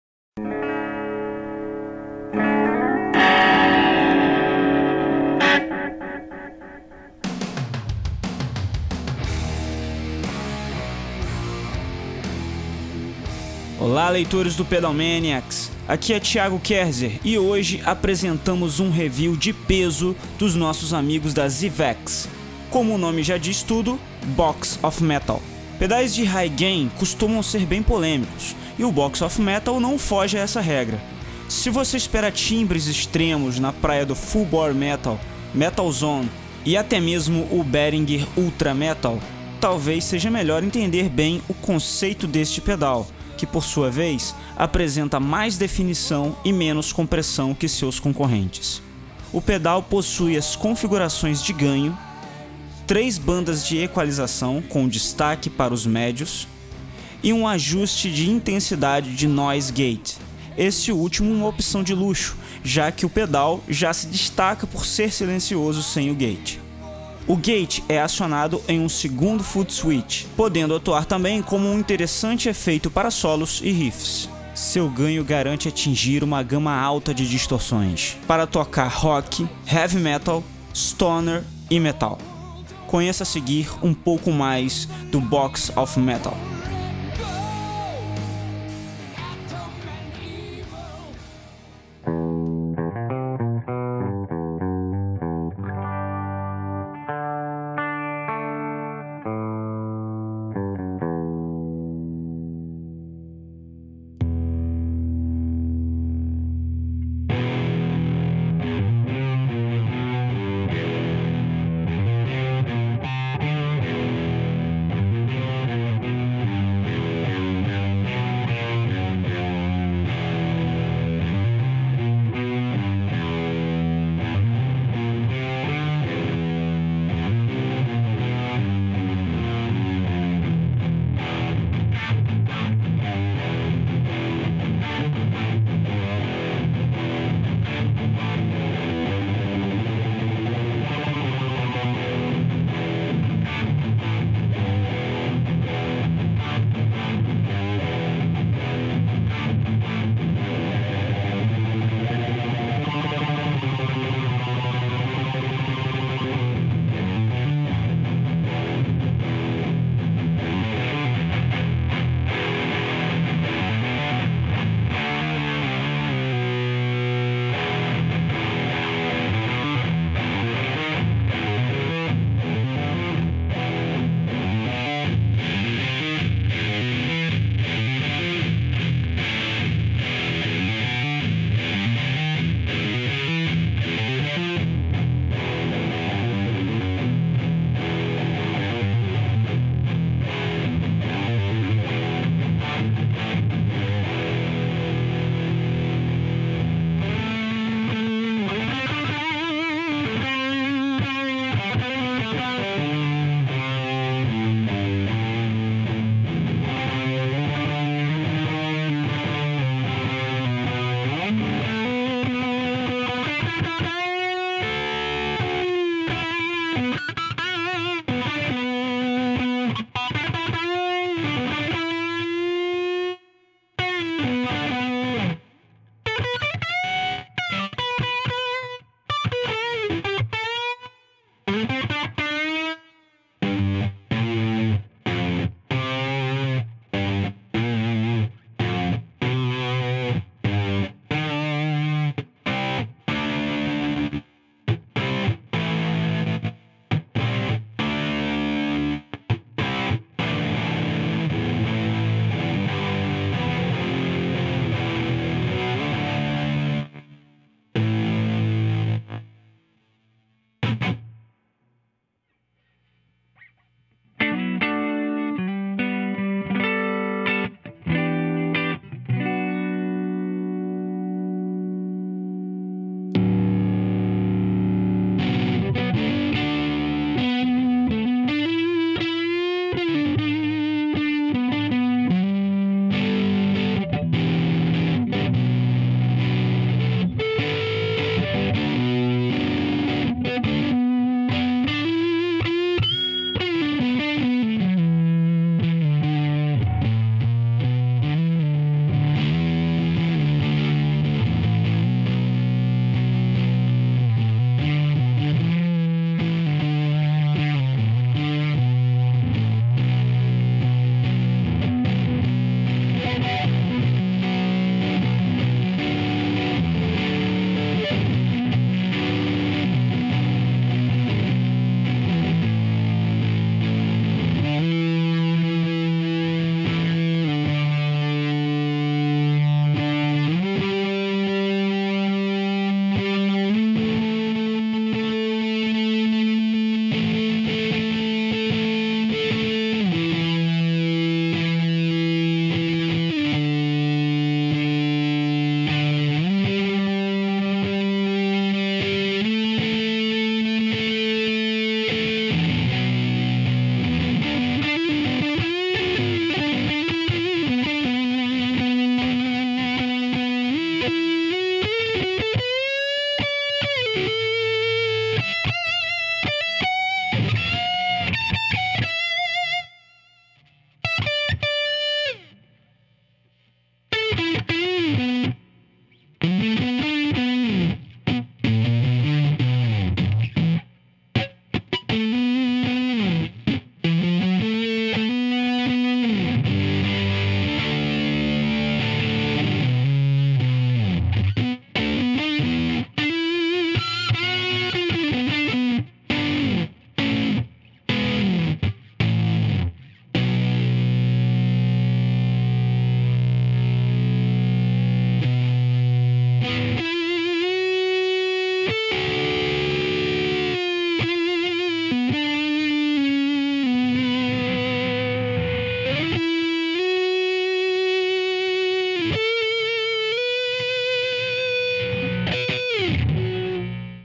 ZVEX - Box Of Metal - Pedalmaniacs
O PedalmaniacsTV anuncia o review de mais um pedal dos nossos amigos da Zvex. Conheça mais sobre o Box of Metal, deixe seu queixo cair e encha-se de GAS ouvindo os timbres e seu maravilhoso GATE!!